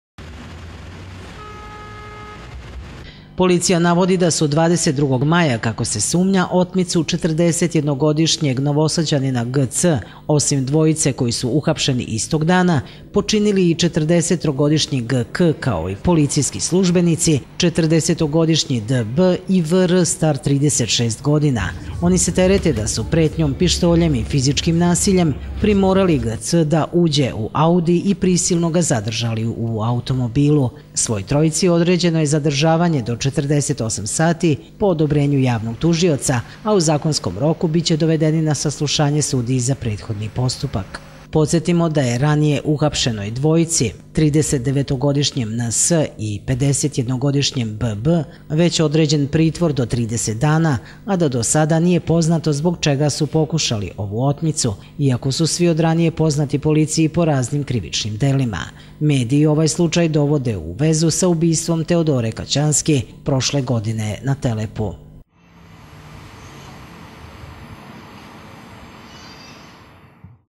TV KANAL 9, NOVI SAD: I POLICAJCI UMEŠANI U OTMICU
Novosadska policija danas je uhapsila je još trojicu muškaraca osumnjičenih da su učestvovali u otmici počinjenoj 22. maja. Među privedenima su i dvojica policajaca.